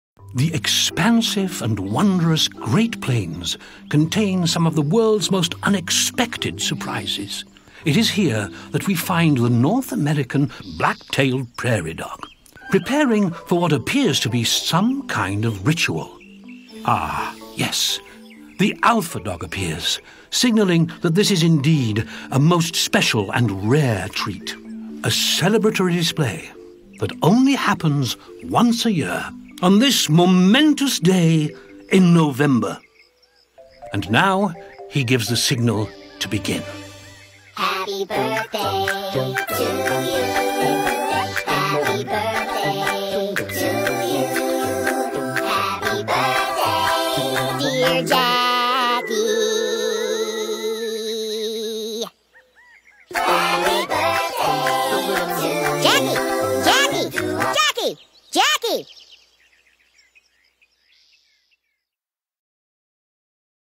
Jackie Peterson's Birthday Prairie Dog BD
The expansive and wondrous Great Plains contain some of the world's most unexpected surprises. It is here that we find the North American Black-Tailed Prairie Dog preparing for what appears to be some kind of ritual. Ah, yes! The alpha dog appears – signaling that this is indeed a most special and rare treat; a celebratory display that only happens once a year, on this momentous day in November!

This video ecard was created at: AmericanGreetings.com